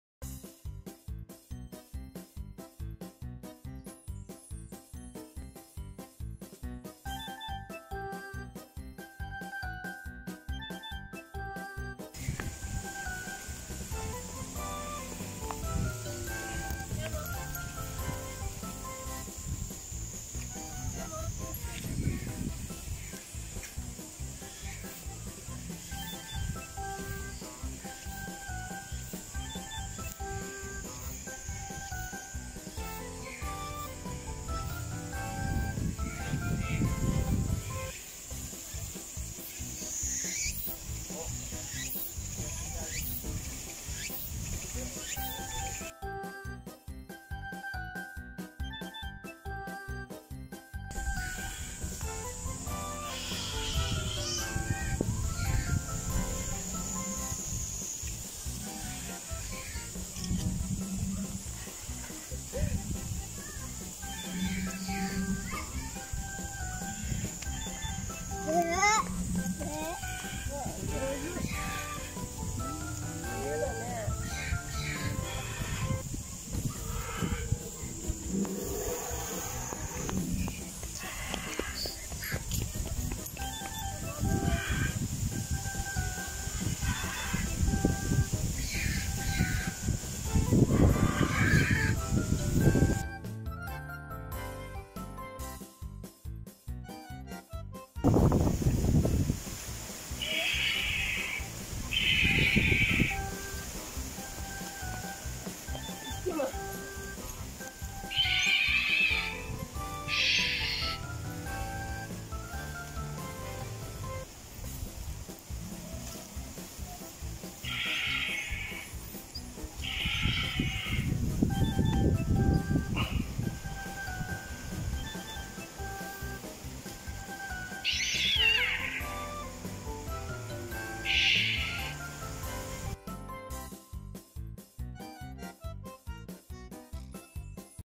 ディノアドベンチャー☆大高緑地☆恐竜探検☆パート2 ☆dinosaur☆ きょうりゅう
恐竜探検パート2です☆

プロトケラトプス＆オヴィラプトル → ヴェロキラプトル＆コンコラプトル → ディロフォサウルス の順番で見ました。

小さな恐竜達がメインです。

引き続きパート3をアップしていきます☆